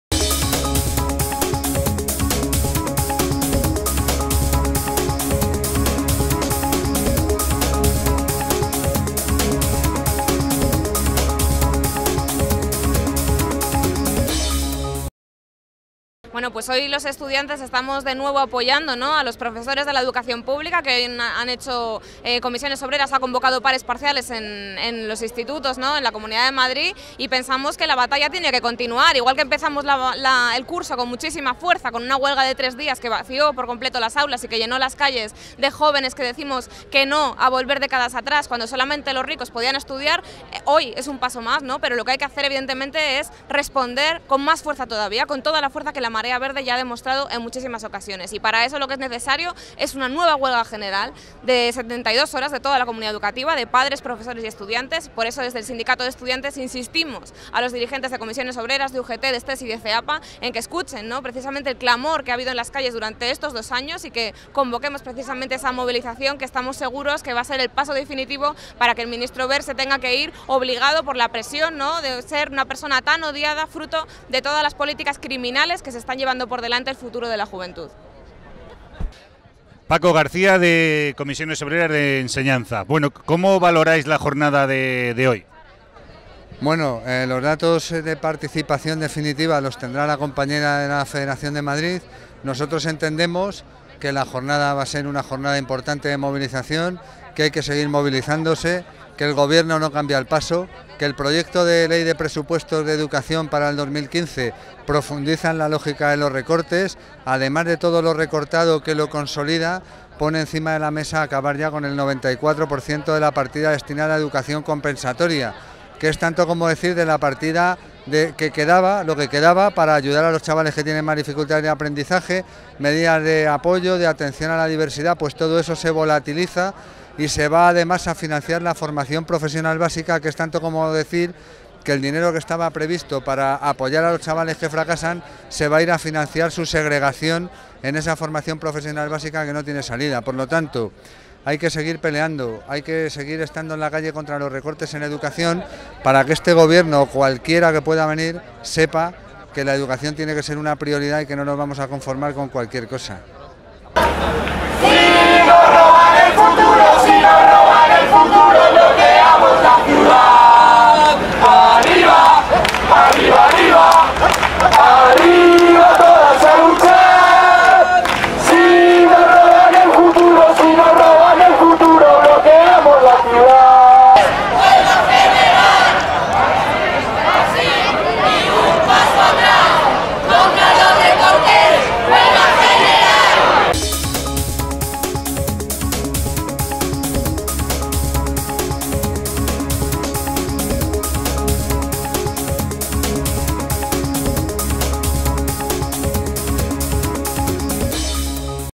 Manifestacion Marea Verde
Manifestación en Madrid contra los recortes en educación y el aumento de las tasas, tras una jornada de huelga y paros parciales del profesorado.